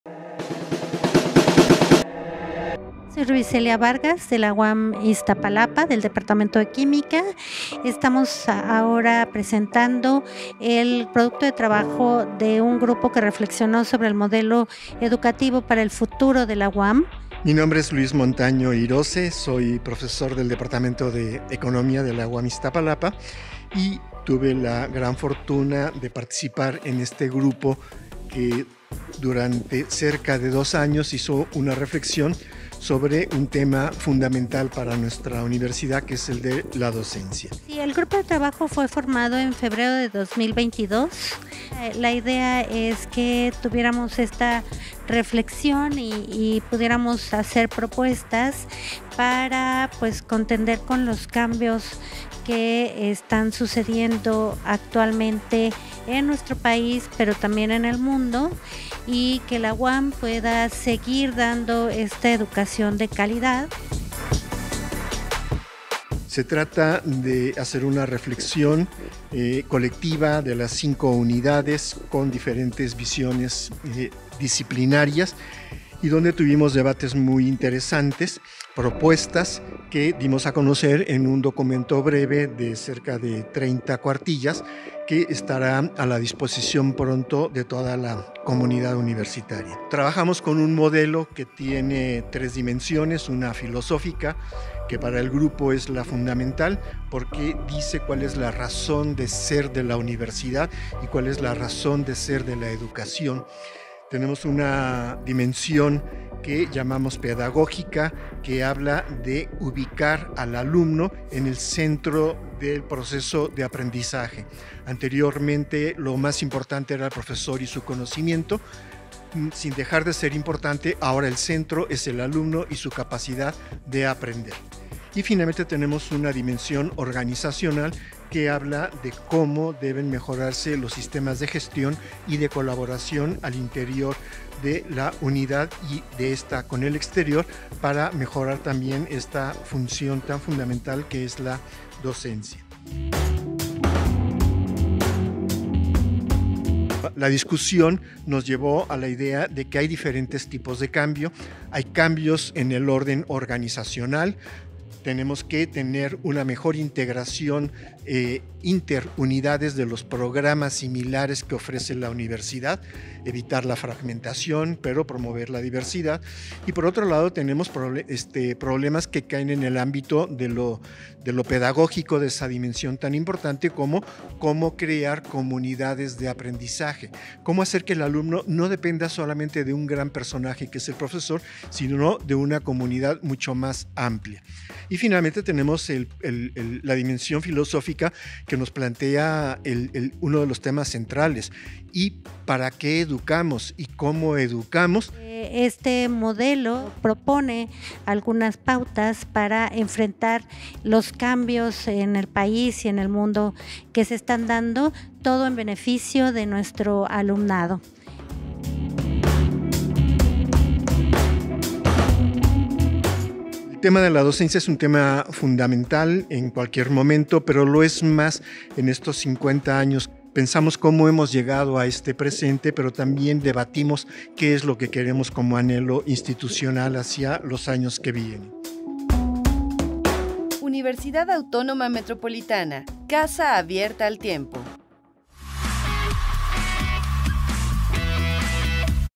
Presentación del Grupo de Trabajo Construyendo el Futuro
Presentación del Grupo de Trabajo Construyendo el Futuro: Presentación del informe del grupo de trabajo de las cinco unidades, Construyendo el futuro. Los desafíos de la Docencia en la Universidad Autónoma Metropolitana.